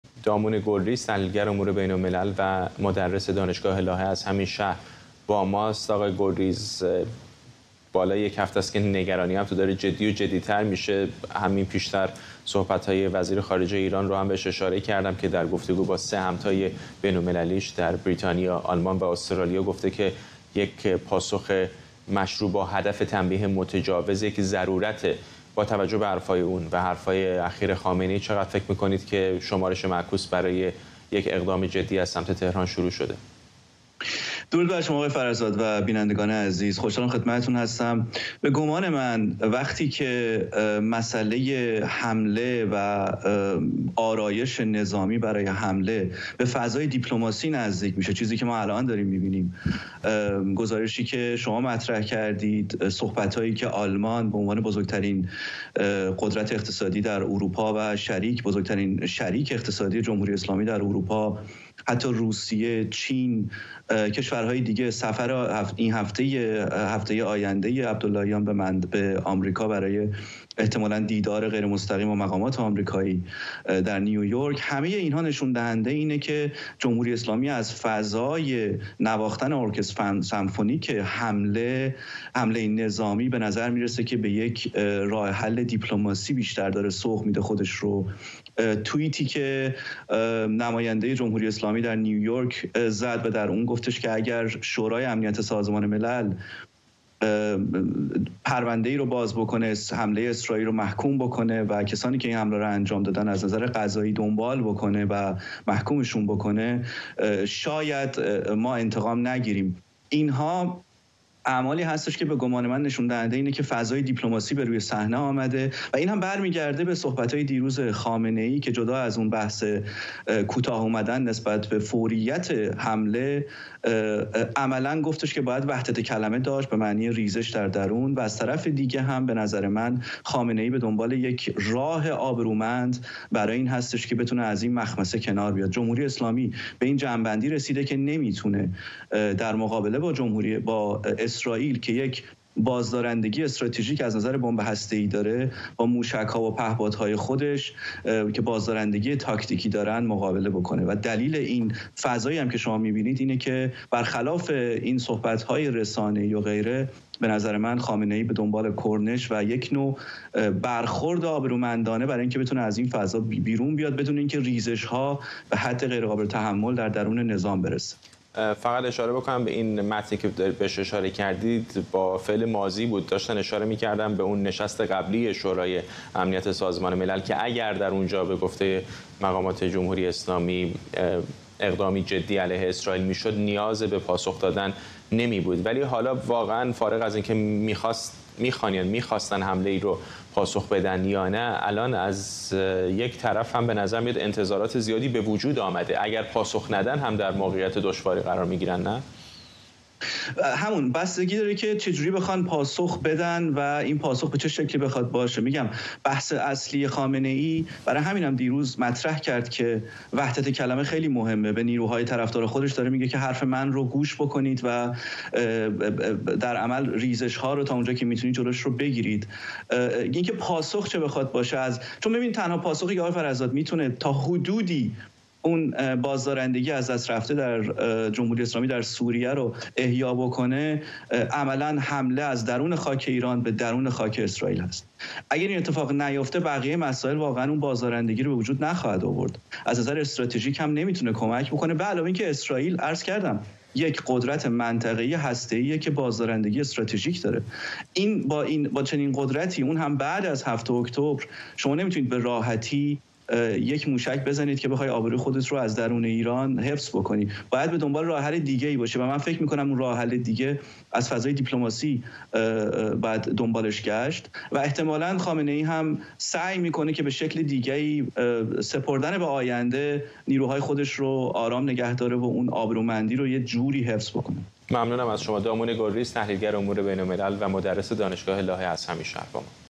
تحقق یا عدم تحقق اقدام جدی نظامی از سمت تهران
دامون گلریز، تحلیلگر امور بین‌الملل: جمیع جهات نشان‌دهنده این است که جمهوری اسلامی دارد از فضای نواختن ارکستر سمفونیک حمله نظامی، خودش را به سمت یک راه حل دیپلماتیک سوق می‌دهد